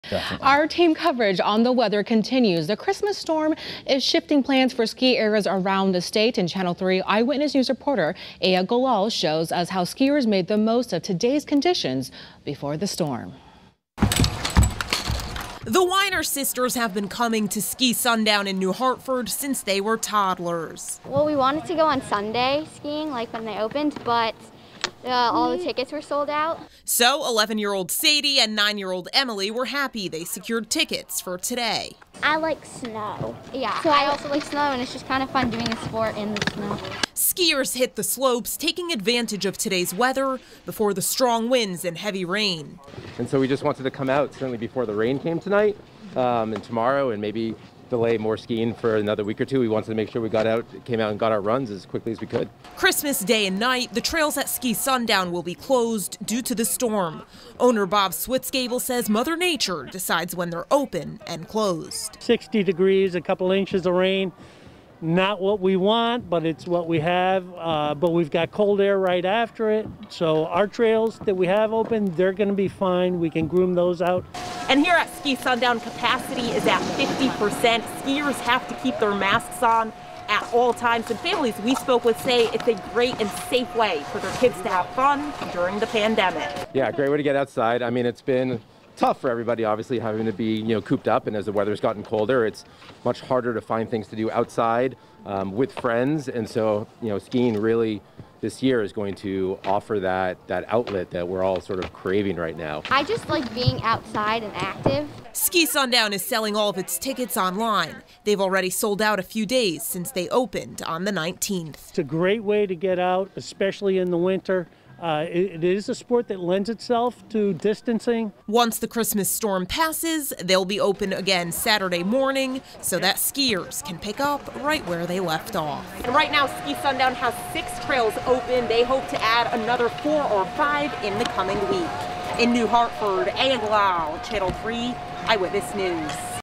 VIDEO: Families hit the slopes before storm hits on Christmas
This Christmas storm is shifting plans for ski areas around the state.